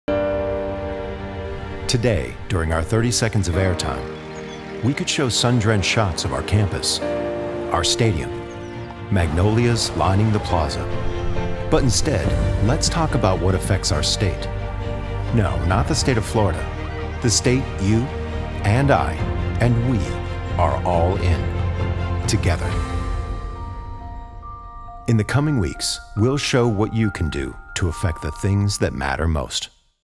For the Gator Good - Teaser - 2014 Television Commercial
Global issues such as cancer, hurricanes, water pollution and hunger can be solved if we, Gators or not, work together. See what you can do for the Gator Good.

Together, our impact is greater.